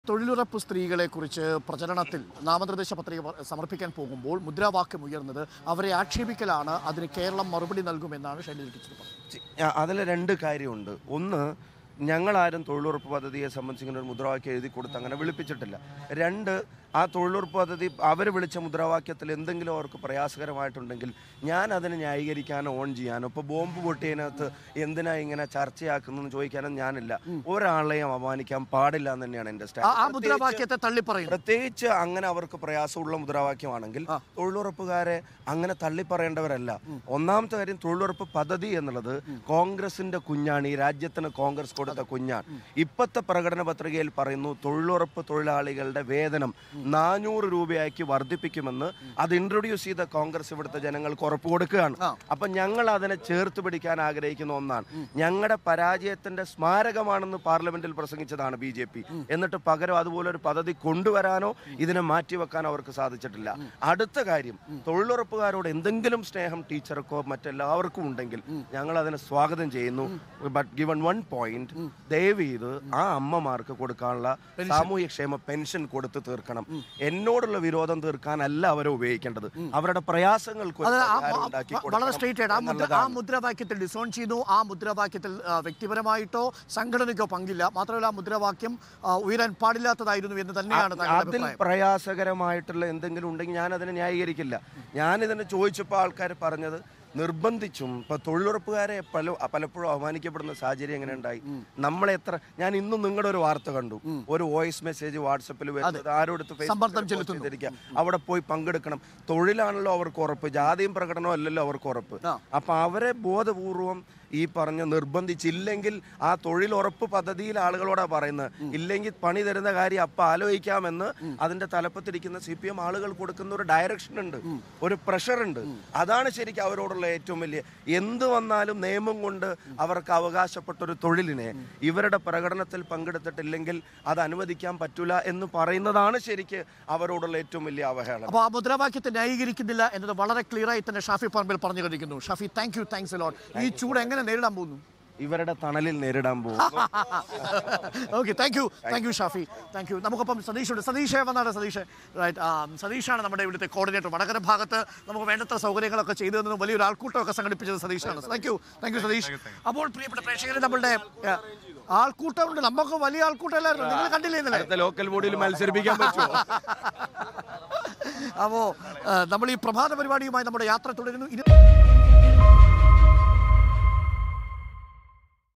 തൊഴിലുറപ്പുകാർക്കെതിരായ മുദ്രാവാക്യത്തെ തള്ളുന്നു | Shafi Parambil | Vadakara
തൊഴിലുറപ്പുകാർക്കെതിരായ മുദ്രാവാക്യത്തെ തള്ളുന്നു, അവരോട് സ്നേഹമുണ്ടെങ്കിൽ ടീച്ചർ പെൻഷൻ കൊടുക്കണം | Shafi Parambil | Vadakara

With Regards
Team RBC